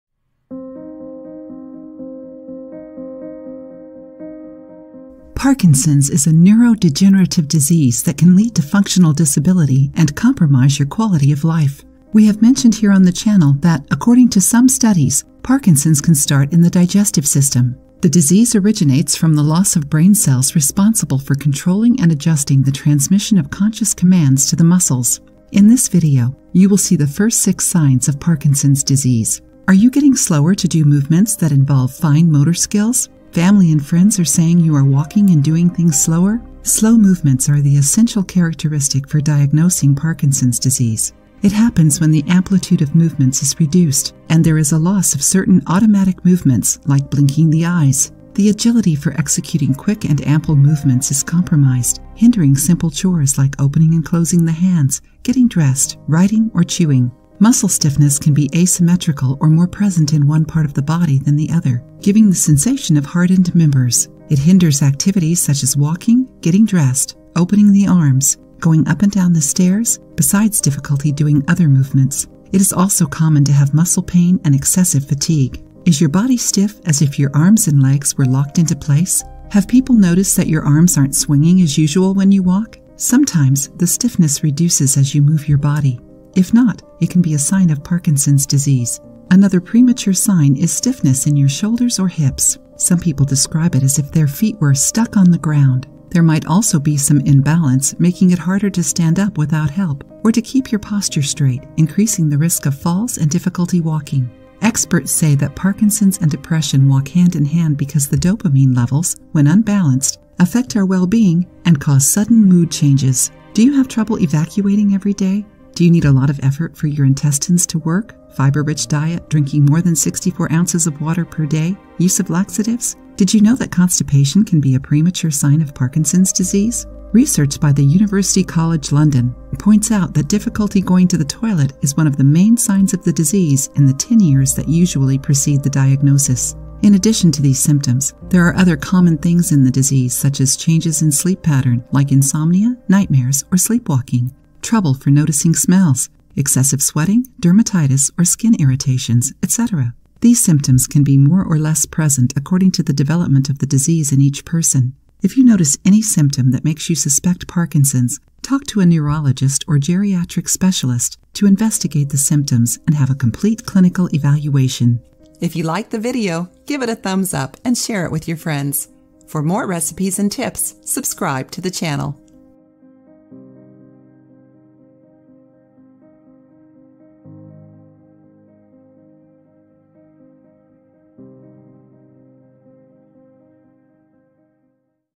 Is Your Body Trying to Tell You Something? 6 Early Signs of Parkinson's
This video discusses Parkinson's Disease, a neurodegenerative disease that can cause functional disability and lower the quality of life.

Studies suggest that Parkinson's can start in the digestive system. The disease is caused by a loss of brain cells responsible for controlling conscious commands to the muscles.

The video goes on to list six early signs of Parkinson's Disease, including slower movements, stiffness in the body, asymmetrical muscle stiffness, depression, constipation, and changes in sleep patterns.

Other symptoms may include excessive sweating, trouble smelling, and skin irritations.

If you notice any of these symptoms, it's best to seek medical advice from a neurologist or geriatric specialist for a complete clinical evaluation.